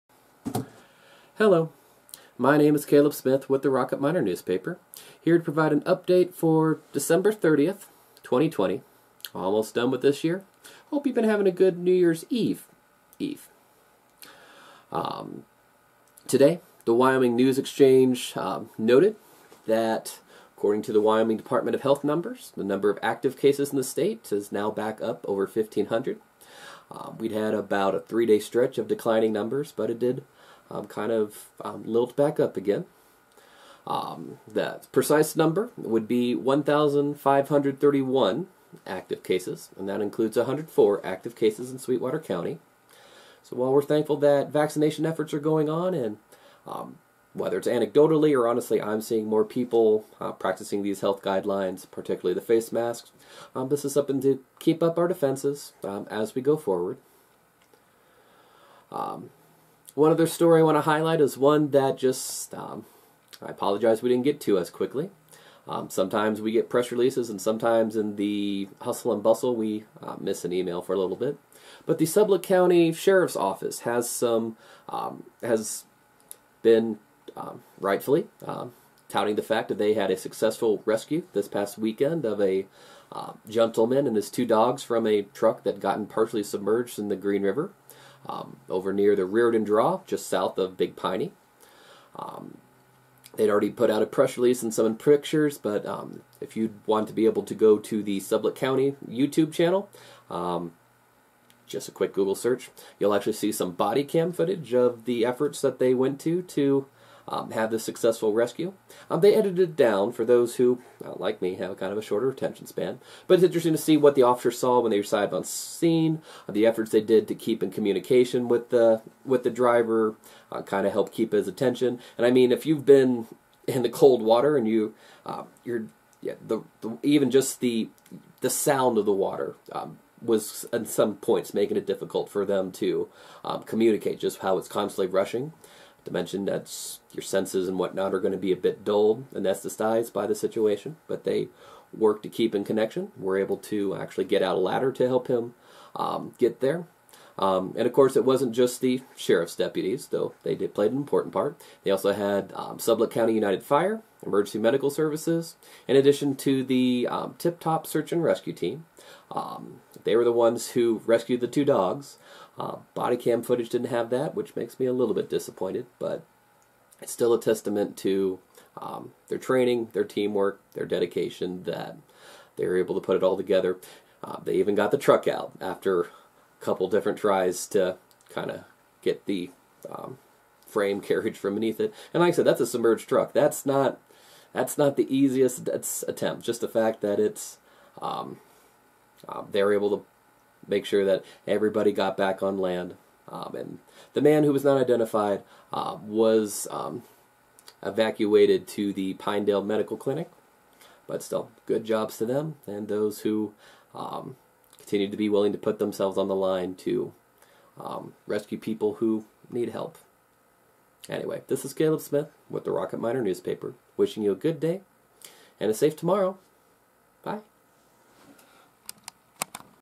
Rocket Miner update: COVID-19 cases rebound; man and dogs rescued from Green River
Wyoming's active coronavirus cases increased to more than 1,500 cases Wednesday. The Sublette County Sheriff’s Office and other first responders successfully pulled a man, two dogs and a truck from the icy waters of the Green River.